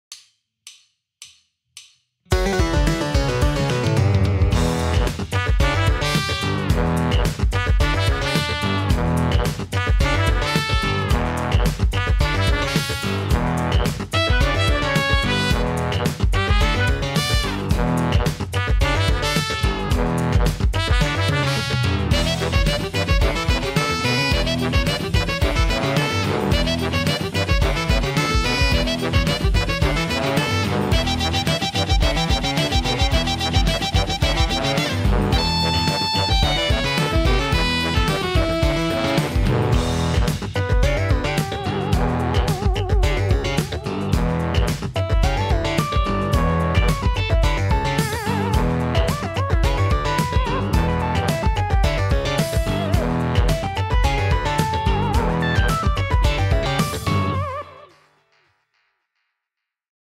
One minute song #1: Funk
I want to make a series of small songs (around one minute long) of different genres.